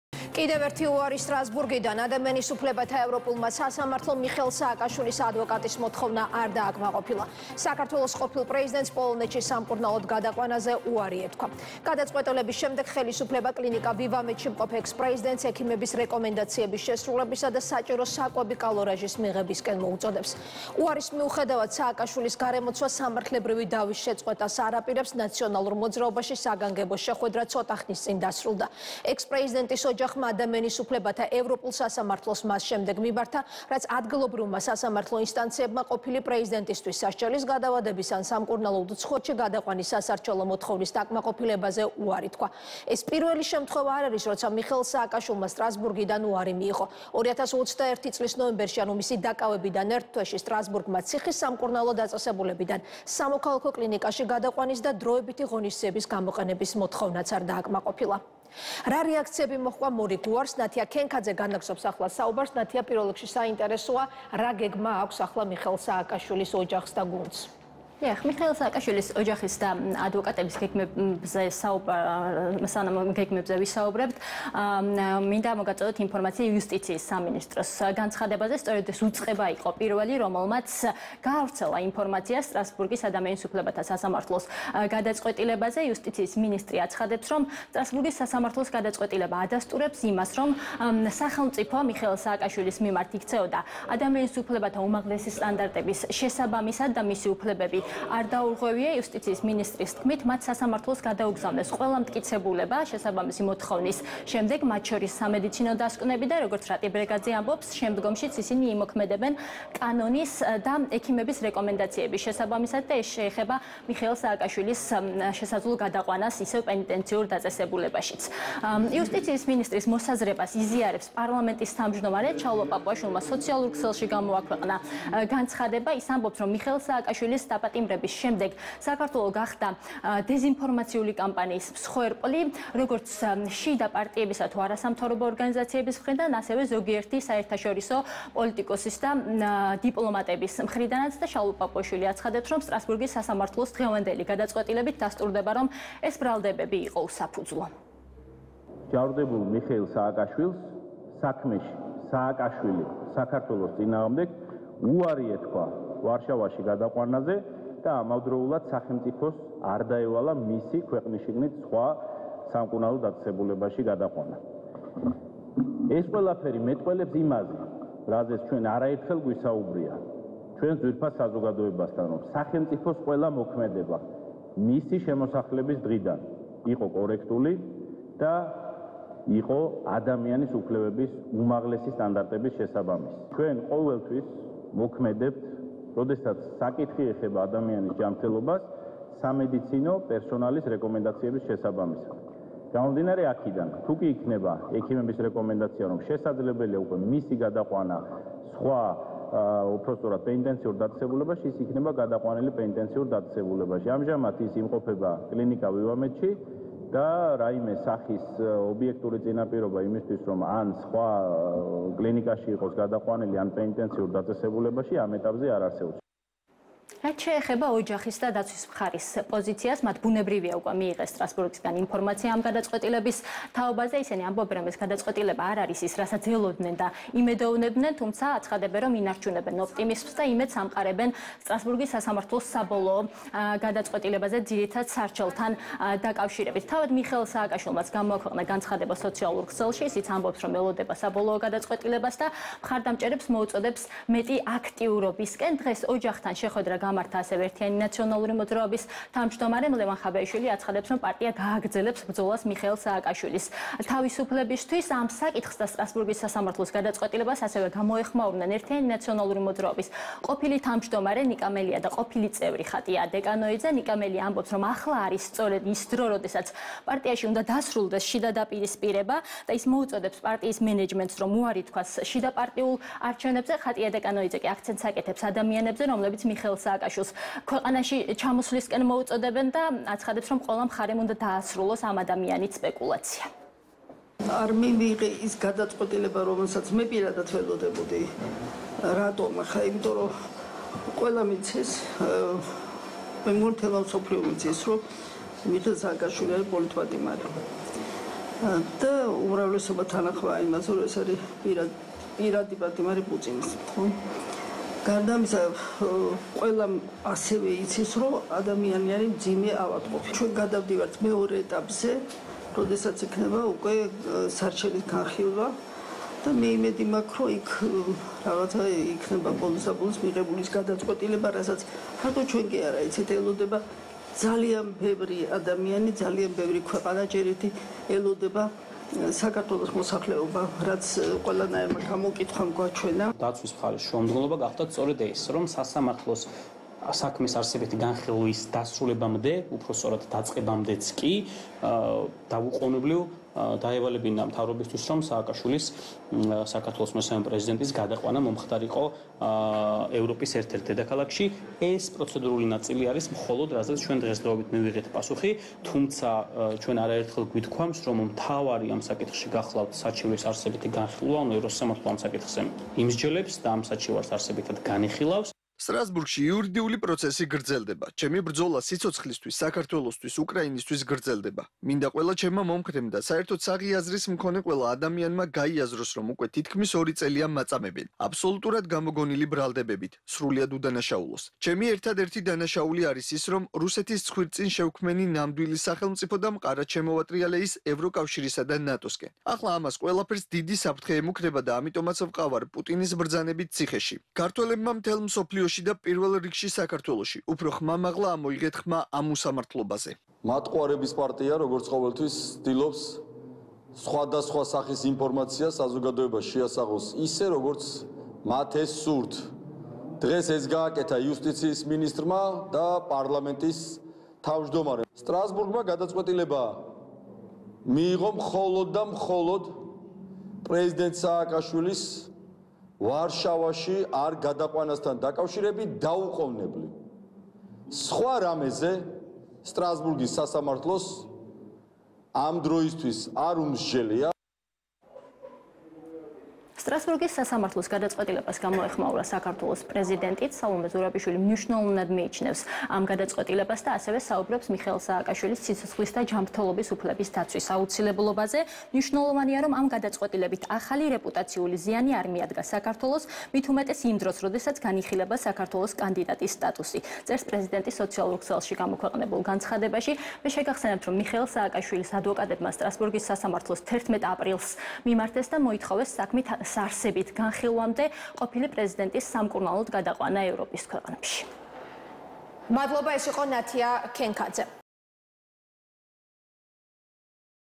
სტრასბურგმა სააკაშვილის მოთხოვნა არ დააკმაყოფილა
კიდევ ერთი უარი სტრასბურგიდან.

ადამიანის უფლებათა ევროპულმა სასამართლომ მიხეილ სააკაშვილის ადვოკატის მოთხოვნა არ დააკმაყოფილა. საქართველოს ყოფილ პრეზიდენტს პოლონეთში სამკურნალოდ გადაყვანაზე უარი ეთქვა. გადაწყვეტილების შემდეგ ხელისუფლება კლინიკა „ვივამედში“ მყოფ ექს-პრეზიდენტს ექიმების რეკომენდაციების შესრულებისა და საჭირო საკვები კალორაჟის მიღებისკენ მოუწოდებს. უარის მიუხედავად, სააკაშვილის გარემოცვა სამართლებრივი დავის გაგრძელებას აანონსებს, „ნაციონალურ მოძრაობაში“ საგანგებო შეხვედრა დასრულდა. ექს-პრეზიდენტის ოჯახმა ადამიანის უფლებათა ევროპულ სასამართლოს მას შემდეგ მიმართა, რაც ადგილობრივმა სასამართლო ინსტანციებმა ყოფილი პრეზიდენტისთვის სასჯელის გადავადების და სამკურნალოდ უცხოეთში გადაყვანის სასარჩელო მოთხოვნის დაკმაყოფილებაზე უარი თქვეს.

ეს პირველი შემთხვევა არ არის, როცა მიხეილ სააკაშვილმა სტრასბურგიდან უარი მიიღო. 2021 წლის ნოემბერში სტრასბურგმა ციხის სამკურნალო დაწესებულებიდან სამოქალაქო კლინიკაში გადაყვანის და დროებითი ღონისძიების გამოყენების მოთხოვნაც არ დააკმაყოფილა.